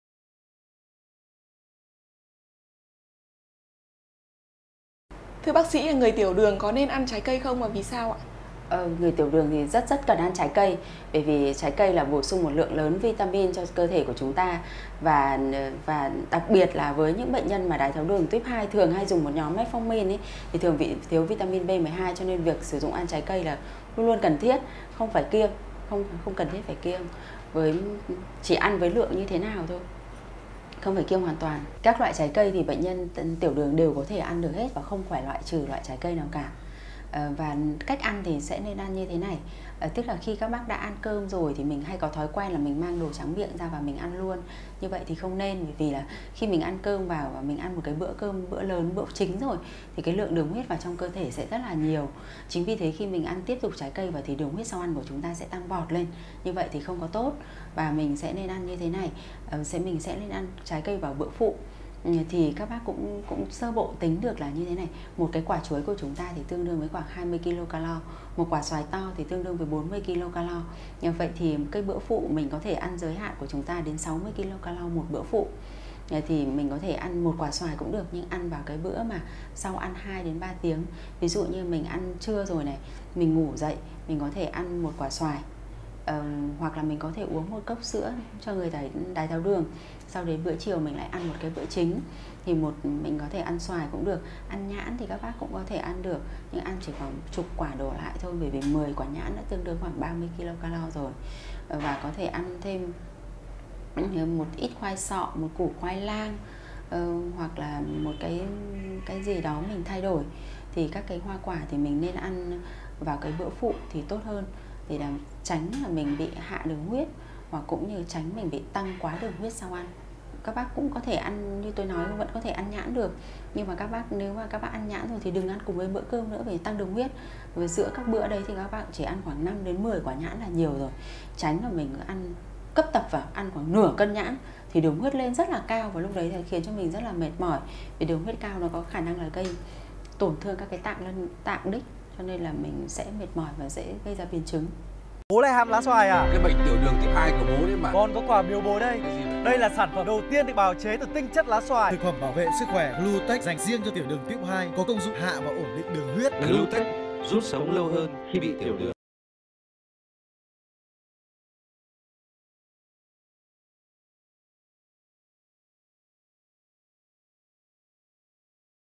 Ăn quả gì để hạ đường huyết | Trái cây cho người tiểu đường
Người bệnh tiểu đường nên ăn trái cây gì ? Ăn quả gì để hạ đường huyết.  Trái cây cho người tiểu đường là nguồn cung cấp vitamin, chất xơ dồi dào cho sức khỏe, nhưng ăn hoa quả không đúng cách rất dễ bị tăng đường huyết.  Vậy bệnh tiểu đường nên ăn trái cây gì ?
Nhận định về cách ăn trái cây cho người tiểu đường đã thay đổi so với  trước kia, đó là bạn có thể ăn bất kỳ loại quả nào mà mình thích, không có sự kiêng khem hoặc không ăn được bất cứ một loại quả nào. Tuy nhiên, theo Ths.Bs Nguyễn Thị Thúy Hằng - Trưởng khoa Khám bệnh, Bệnh viện Đa khoa Xanh Pôn, Hà Nội, trái cây cũng có chứa một lượng đường nhất định, vì vậy nếu ăn quá nhiều hoặc không đúng thời điểm sẽ làm tăng đường huyết lúc đói, góp phần làm tăng HbA1c. Đây là điều tối kỵ với người bệnh tiểu đường. Vì lẽ đó mà trái cây nên được ăn trong bữa phụ (cách bữa chính 2 - 3 tiếng) và tùy thuộc vào lượng calo của loại hoa quả, người bệnh cần đánh giá được loại nào nên ăn nhiều, loại nào ăn ít.
 Nếu bạn có bất kỳ boăn khoăn nào cần tư vấn về bệnh tiểu đường, chế độ ăn - uống, tập luyện và dùng thuốc bạn có thể gọi tới số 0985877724 để được tư vấn.